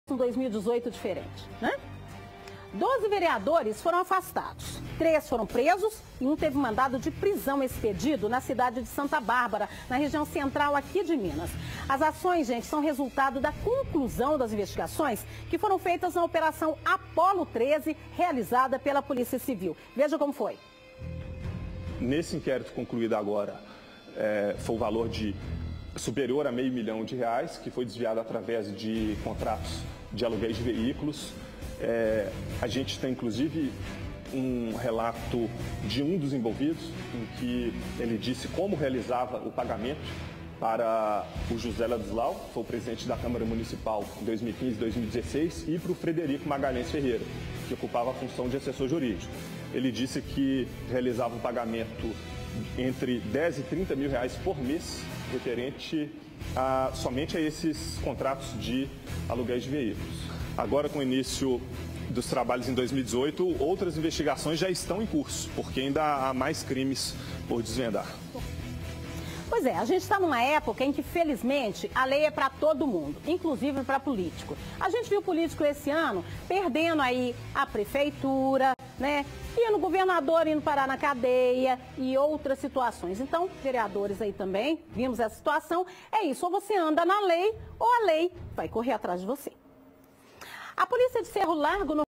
Investigação afasta 12 vereadores de Santa Bárbara
Além dos parlamentares afastados, outros três foram presos e um teve mandado de prisão expedido.